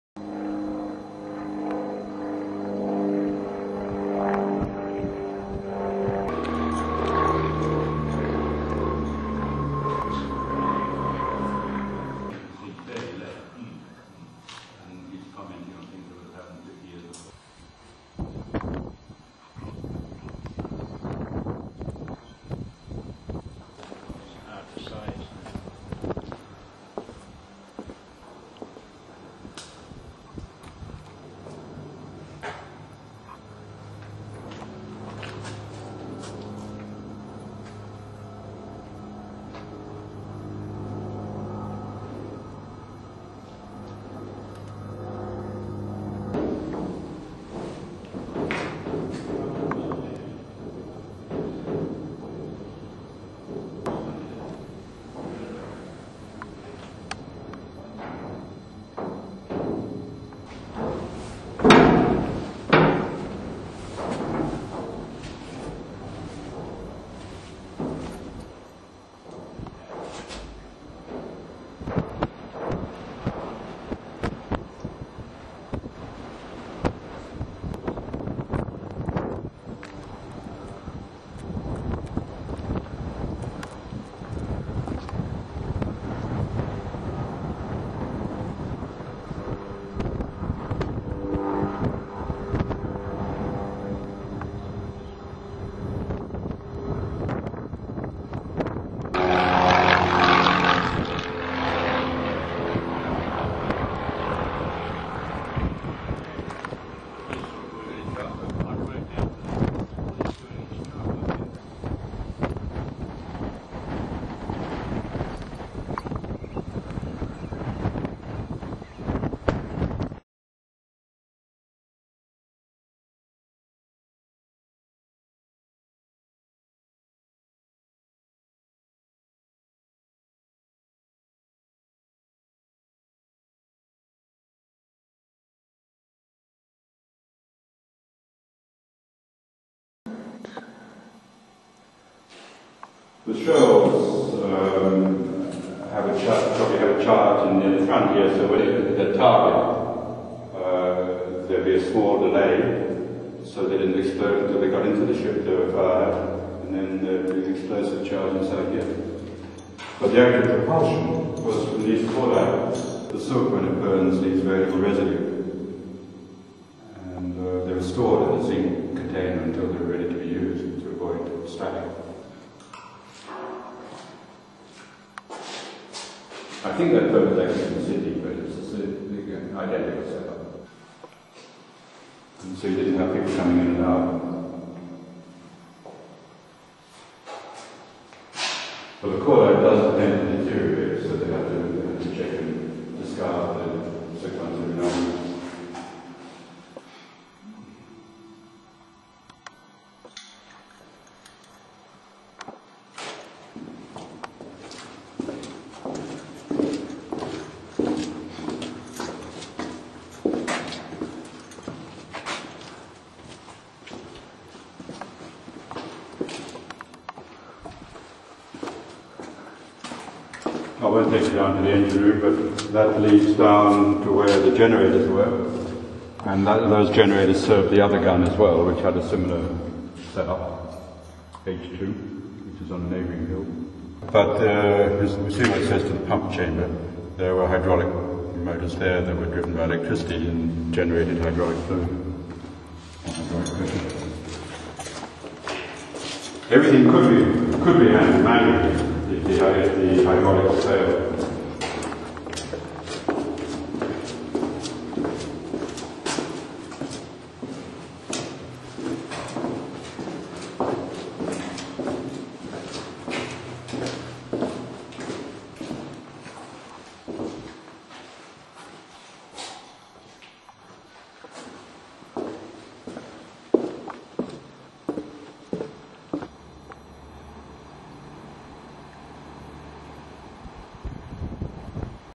Oliver Hill Gun Rottnest Island
The restored gun enplacement with underground bunkers and tunnels under Oliver Hill